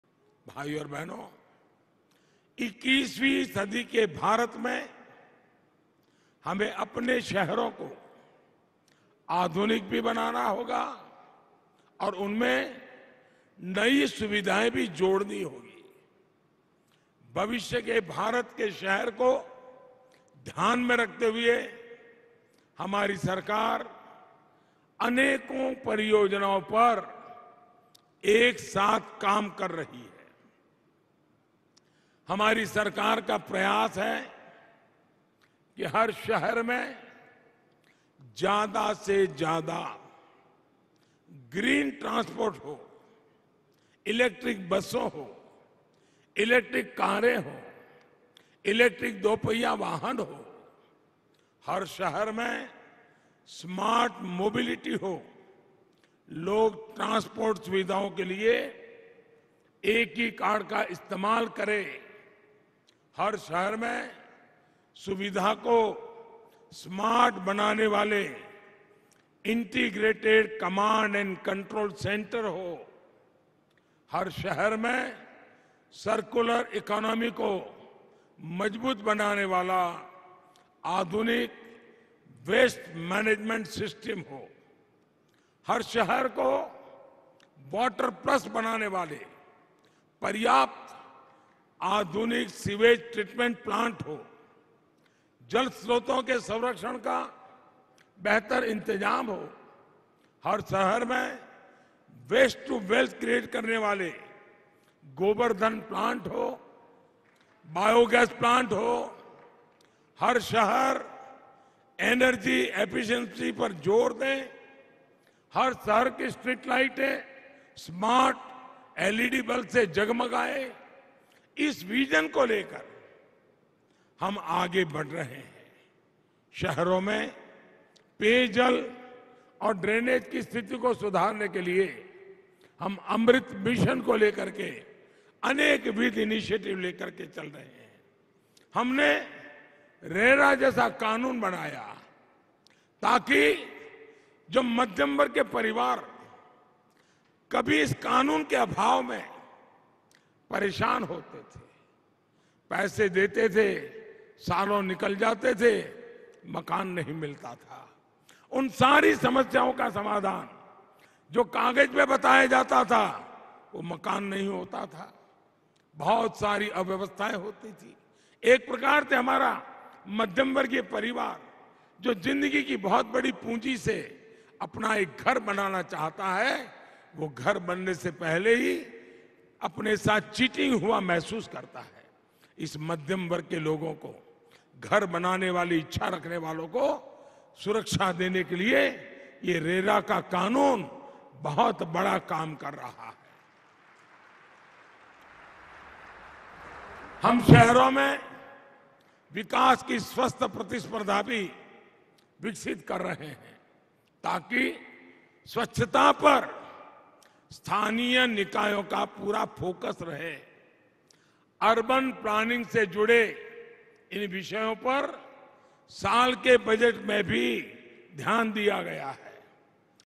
Our aim is to ensure smart mobility for the people: PM Modi
PM Narendra Modi inaugurated Pune Metro Rail Project and laid the foundation stone and inaugurated multiple developmental projects in Pune. PM Modi said, “It is my good fortune that you had invited me to lay the foundation stone of Pune Metro and now you have given me the opportunity to inaugurate it. It also has the message that plans can be completed on time.”